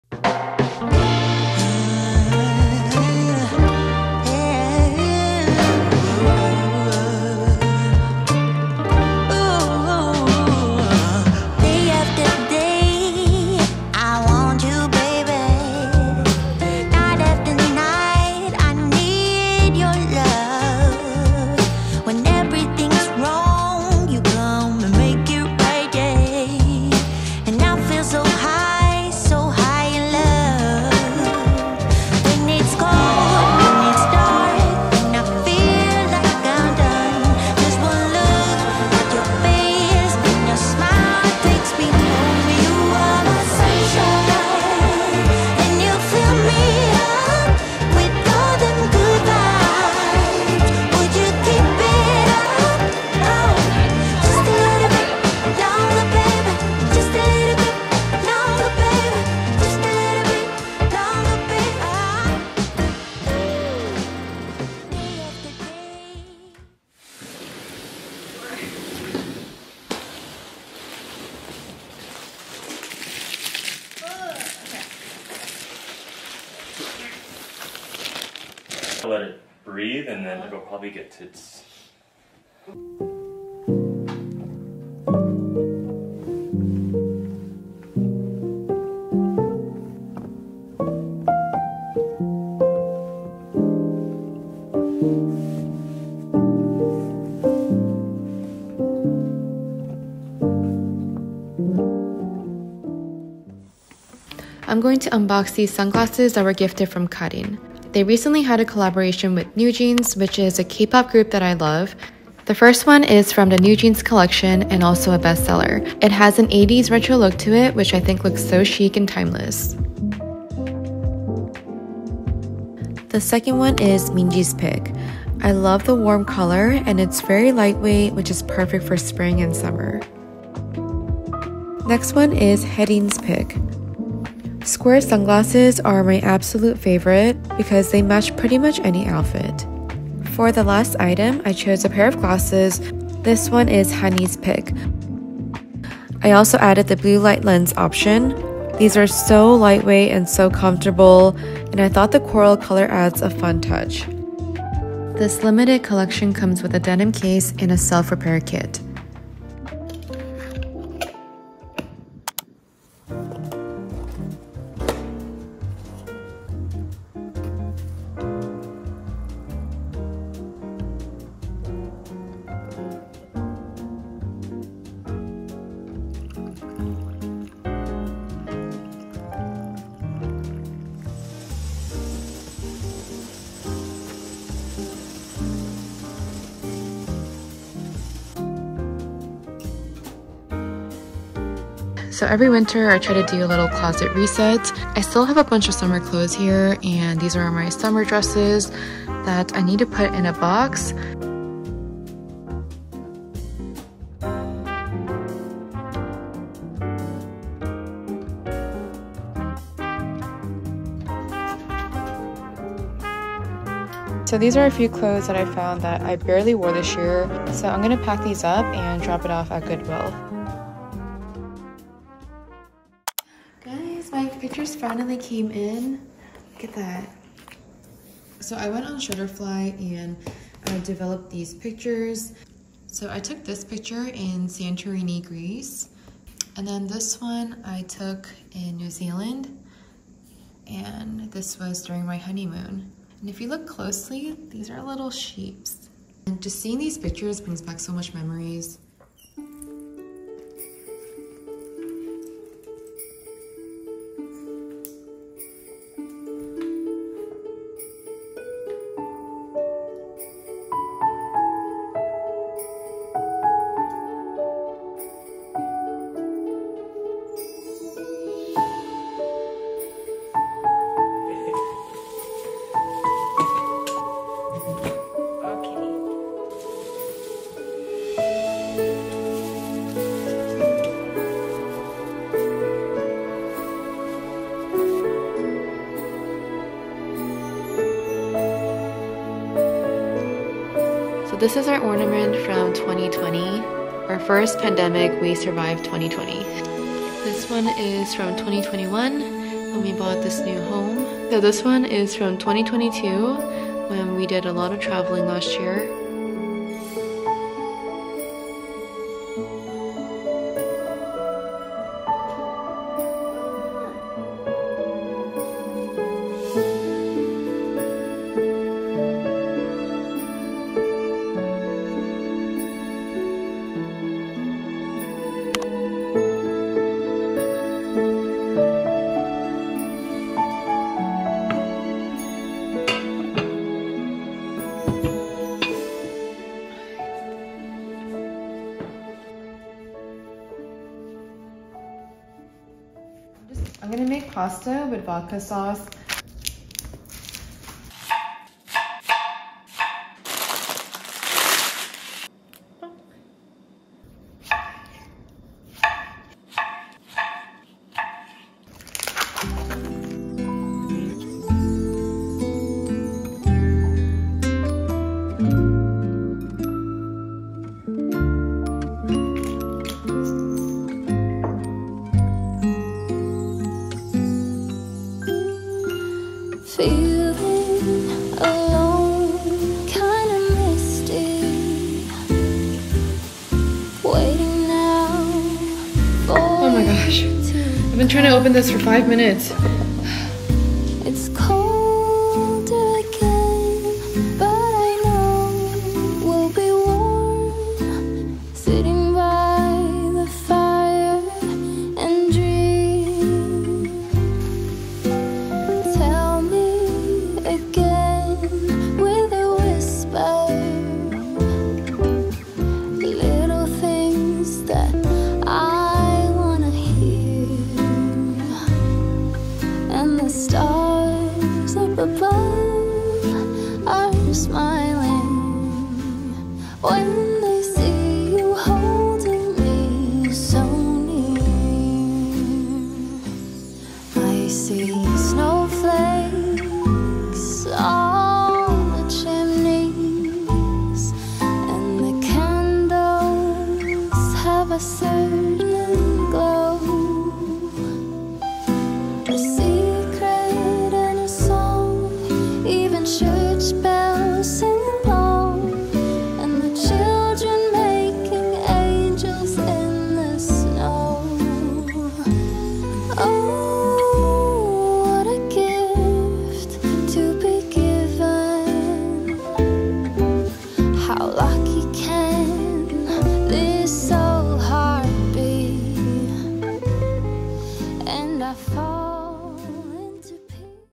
Getting ready for Christmas, NewJeans Sunglasses, Cozy Tree decorating
Nectar King Mattress

Hi guys! It's the holiday season and Christmas is coming up. In this vlog, I get ready for Christmas and decorate my Christmas tree, try out my new bed from Nectar, and unbox the New Jeans Collection sunglasses from the CARIN collaboration. I can't believe the year is almost coming to an end and I am super excited for the holiday season and the new year. My plan is to create more cozy and comforting vlogs for you guys to enjoy this holiday season.